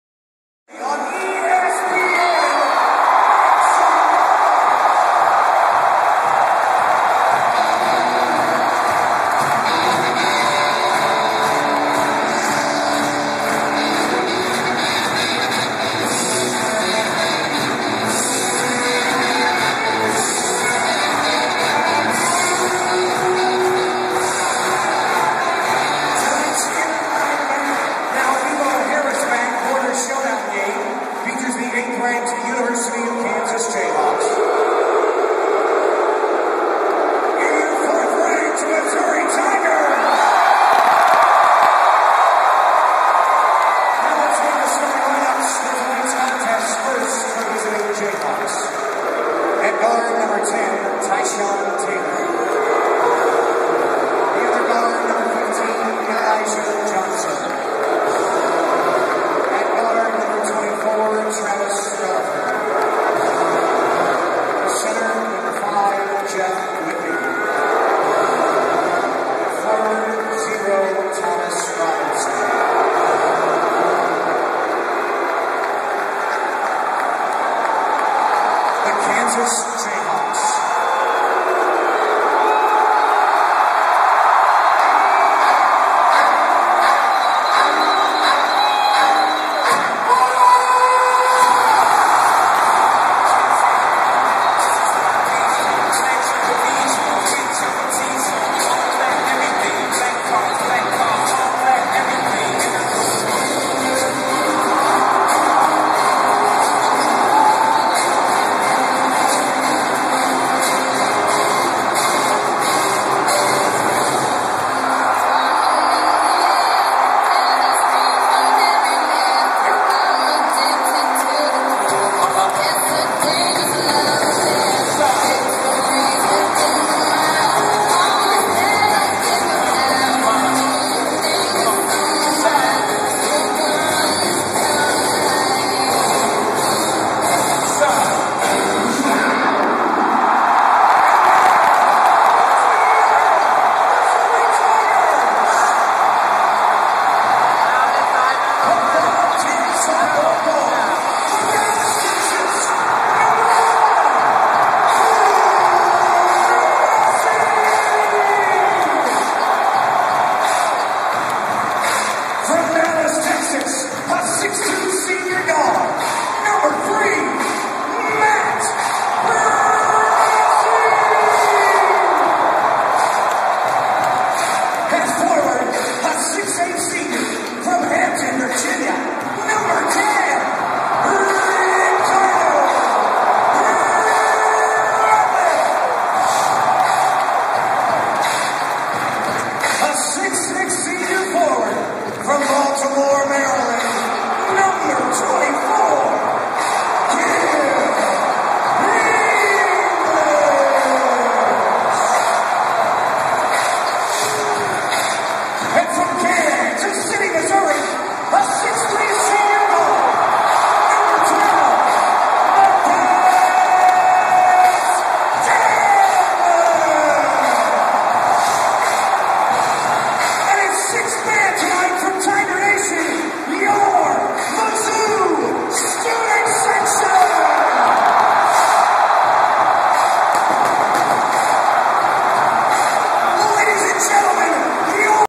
Missouri/Kansas Basketball Introductions, Feb. 4, 2012
Pre-game starting lineups and intro video (Mizzou Arena, Columbia, Mo.)